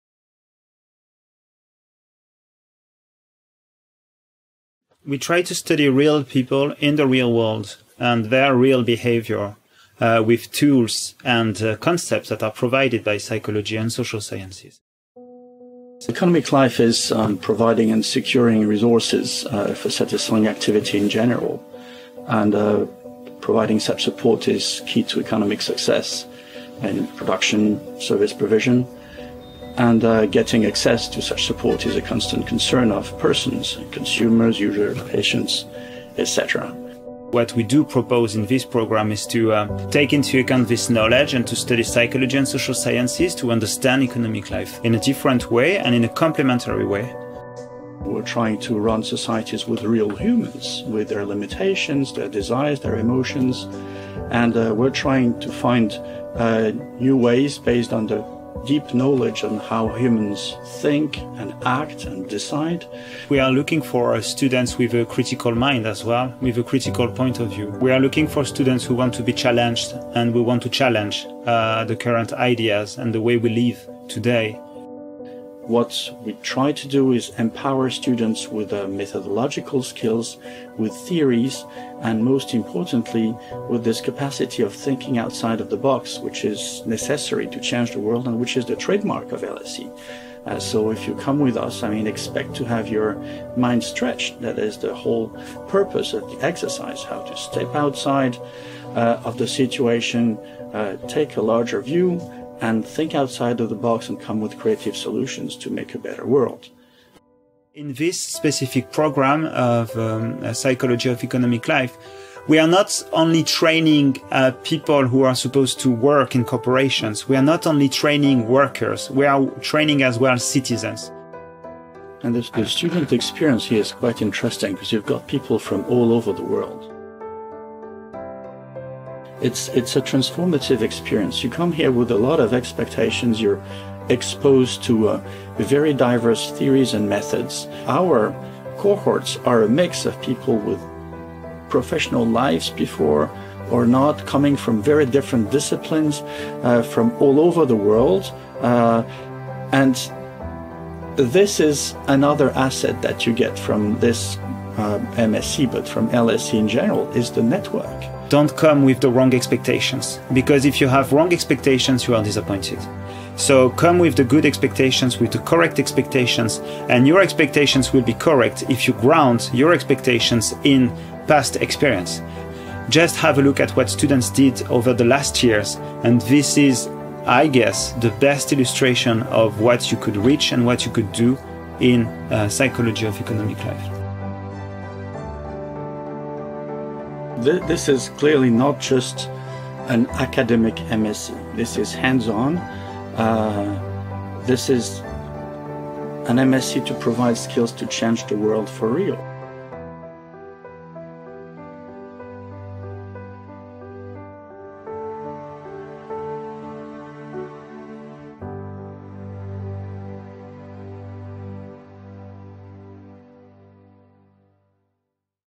MSc Psychology of Economic Life
Explore the nature of economic behaviour and thought. Listen to faculty and students from the Department of Psychological and Behavioural Science talk about MSc Psychology of Economic Life and what you could expect from this postgraduate programme at LSE.